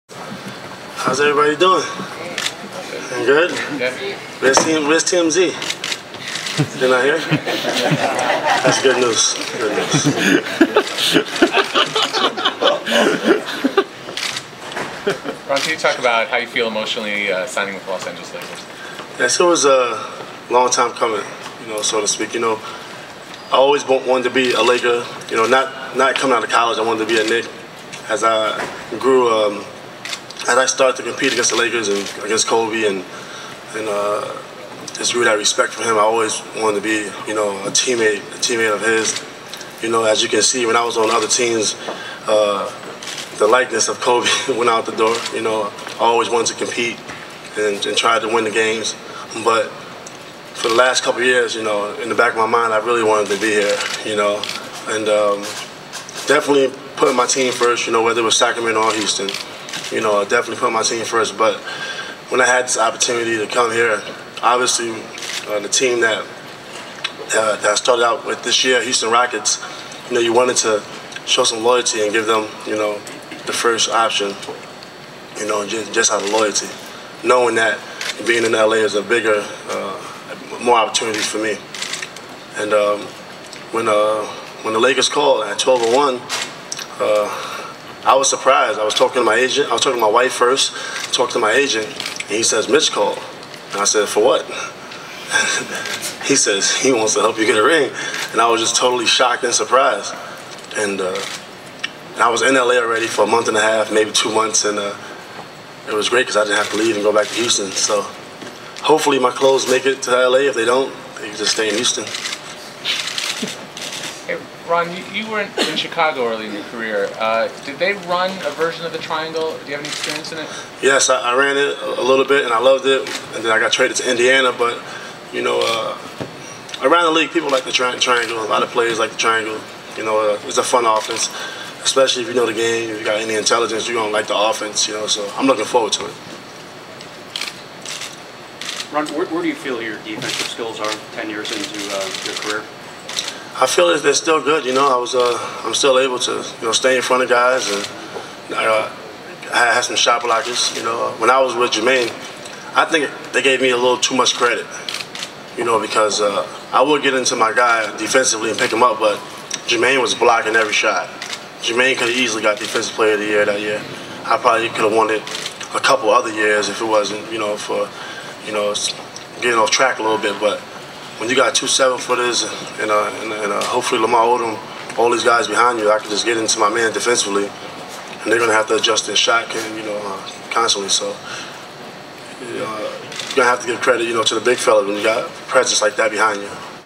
Ron Artest, on being in L.A., the triangle and defense
During his press conference, new Laker Ron Artest talks about his happiness to be in Los Angeles, the triangle offense and his defensive skills.